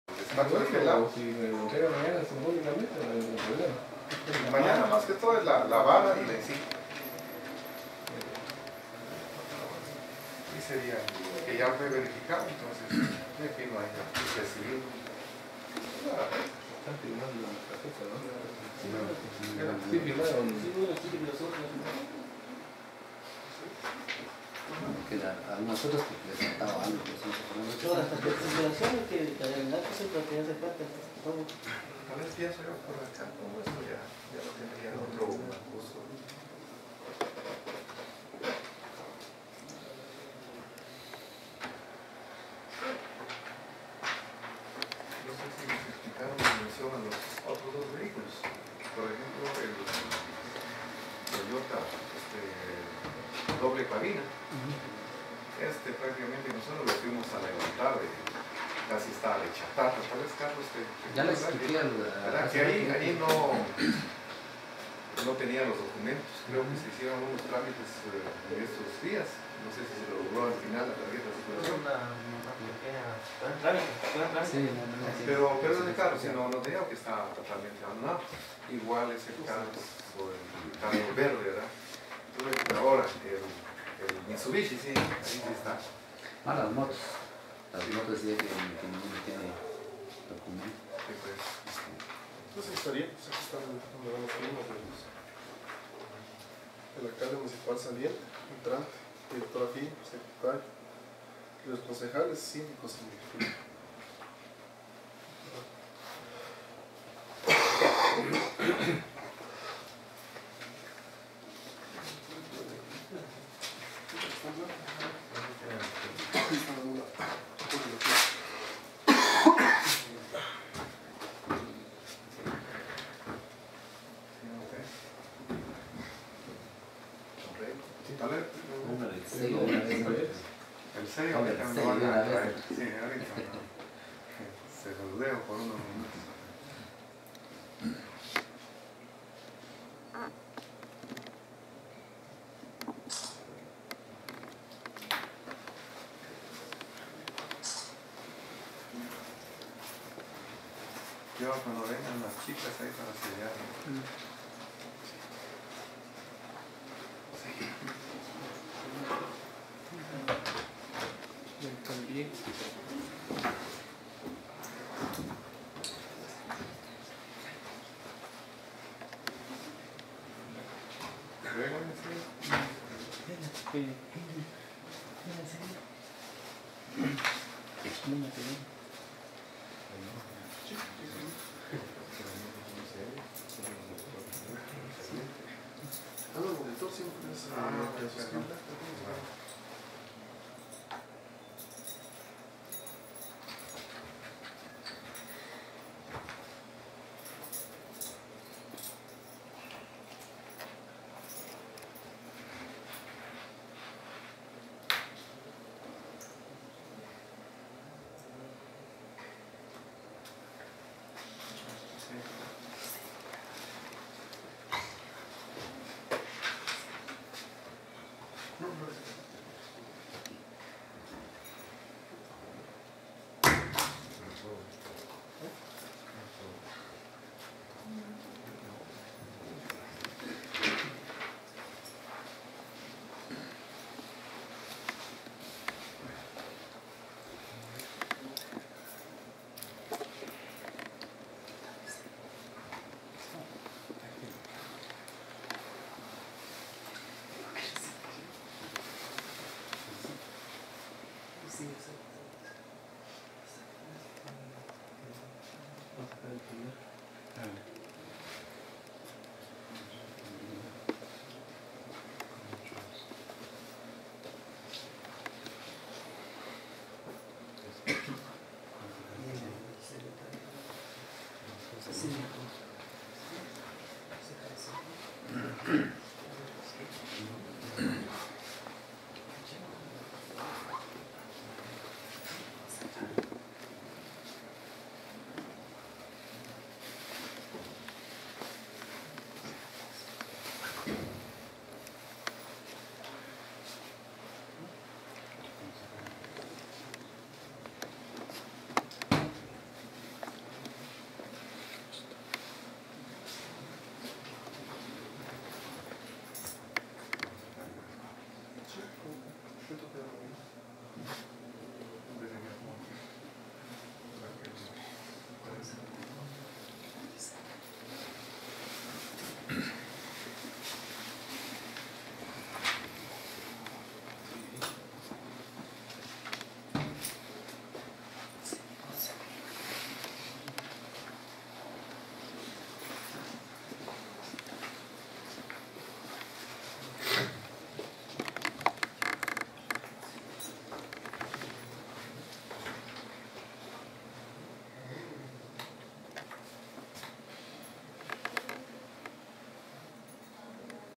Entrega de llaves por el Alcalde saliente
Patzún, Entrega de llaves por el Alcalde saliente en el proceso de transición de las nuevas autoridades municipales con la toma de posesión del Alcalde Reyes Patal Yos